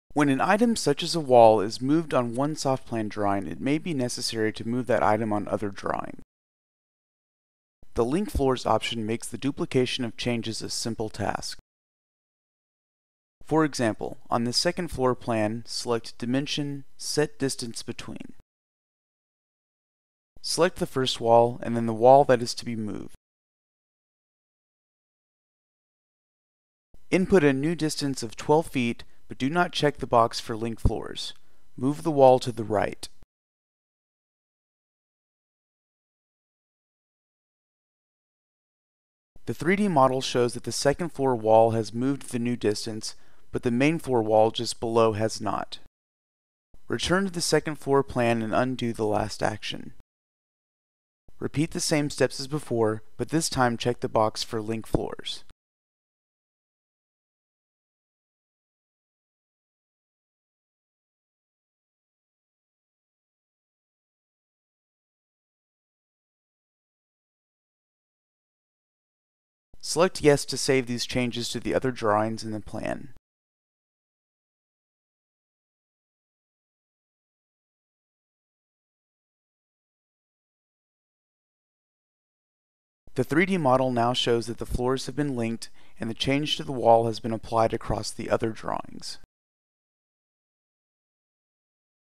Link Floors
SoftPlan Home Design Software: When an item is moved that affects two floor plan drawings, use Link Floors to duplicate the change.